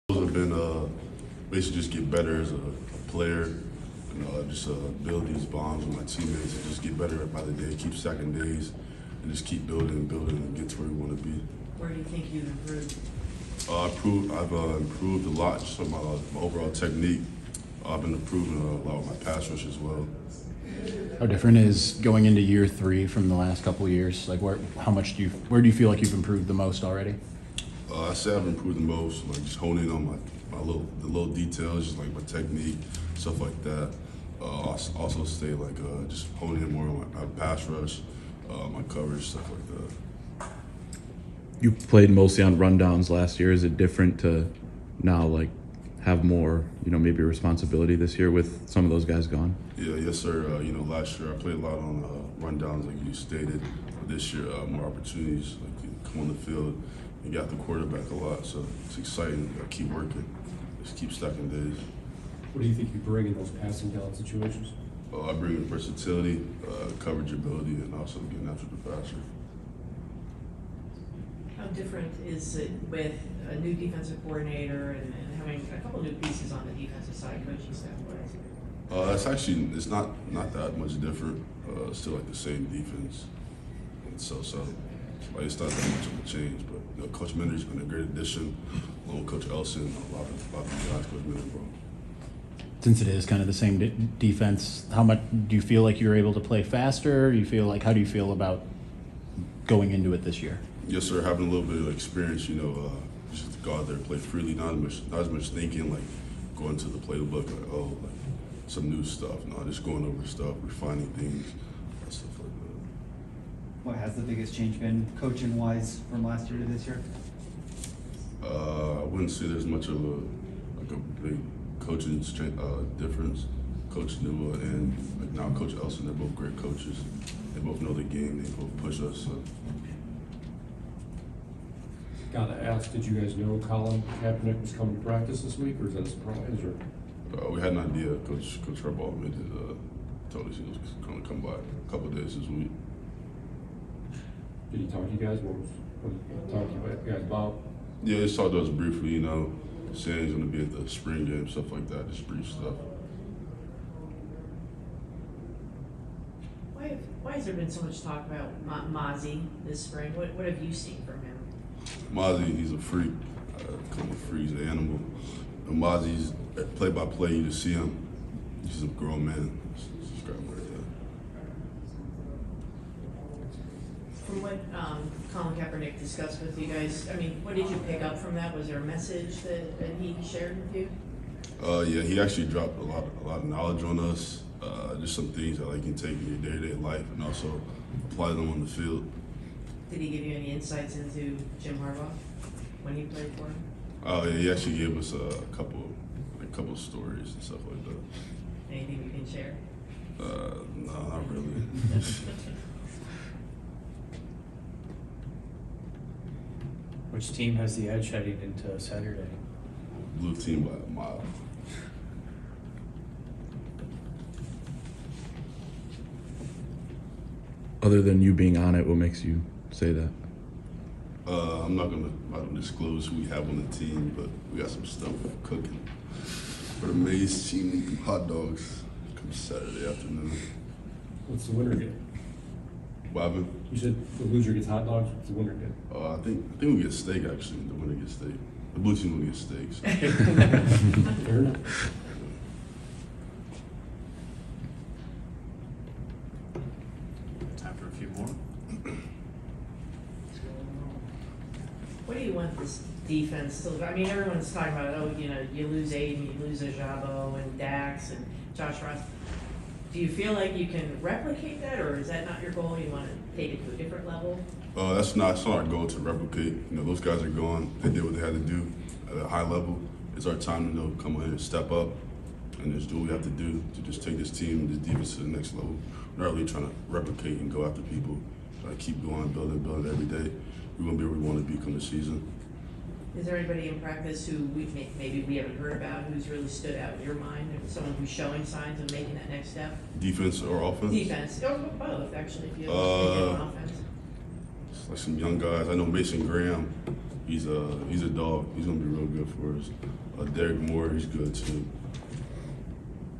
Michigan OLB Jaylen Harrell Discusses His Increased Role, Development
Michigan football OLB Jaylen Harrell — March 32, 2022